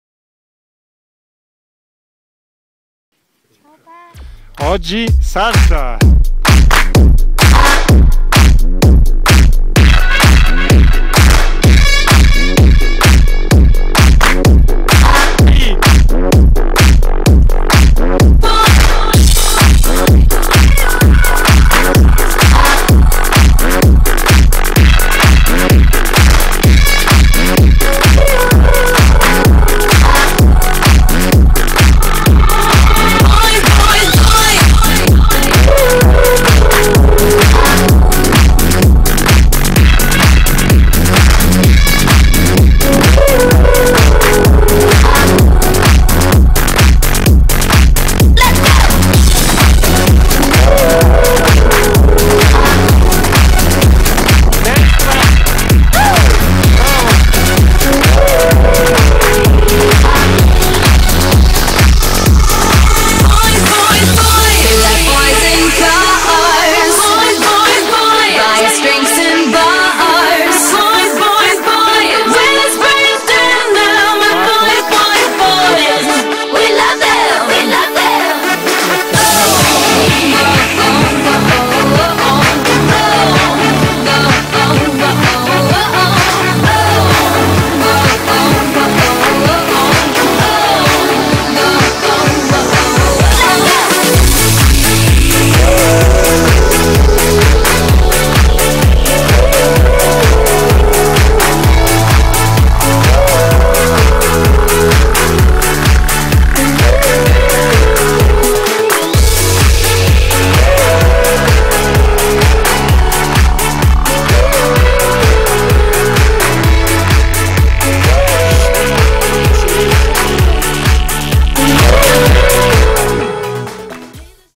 La salza
Iacopo scia fuoripista sulla Salza